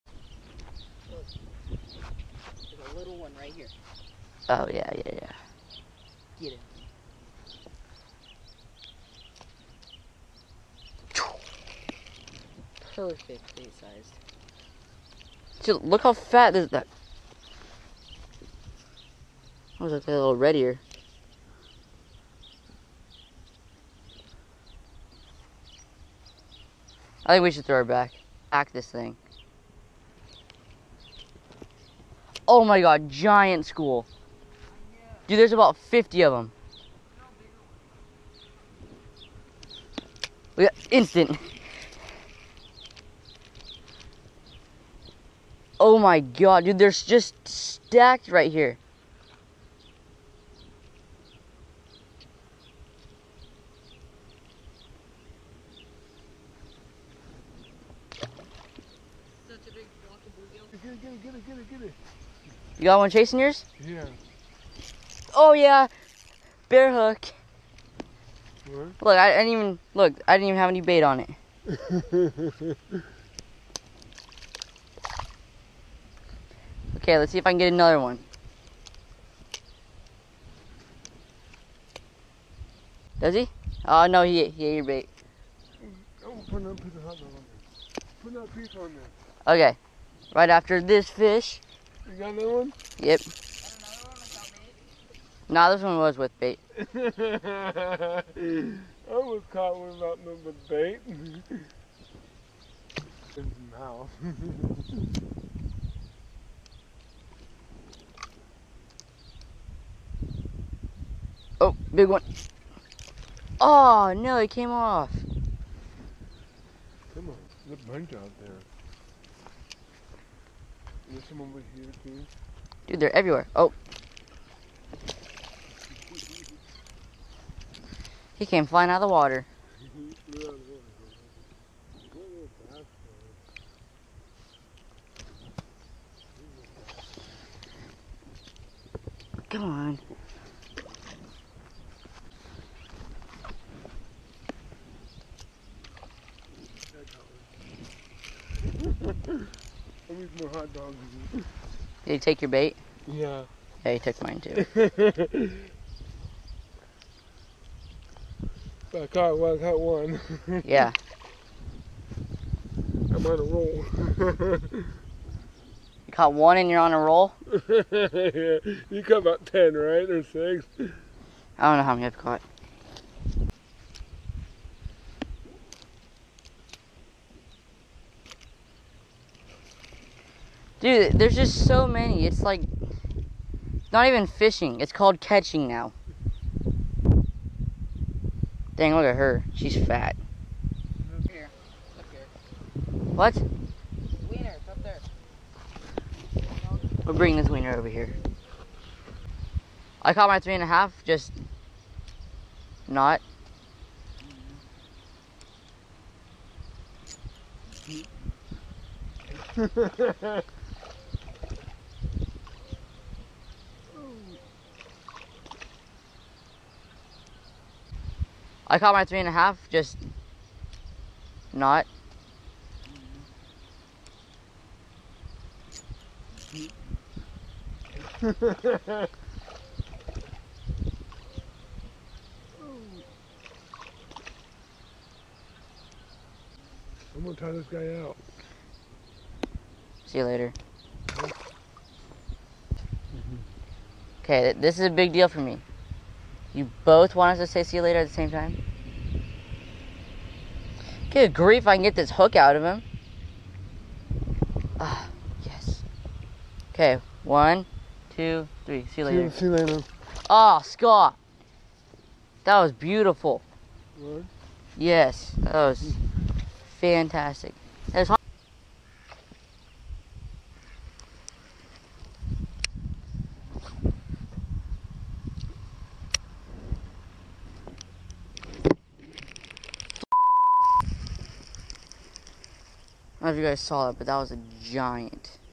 Bluegill Fishing With No Bait?
bluegill fishing with no bait? we caught 2 with no bait, but the rest we mostly caught on hotdogs.
use justkidsfishing for shout out on instagram or oooohmypatchia